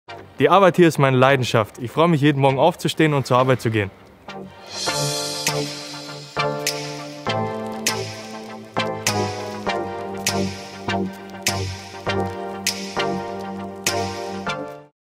Werde Baustellenleiter • Louis 2 • Walker Sindelfingen
"Die Arbeit bei der Walker in Sindelfingen ist meine Leidenschaft." Du willst dich auf jeden Morgen auf deine Arbeit freuen? Dann bewirb dich bei uns als Baustellenleiter.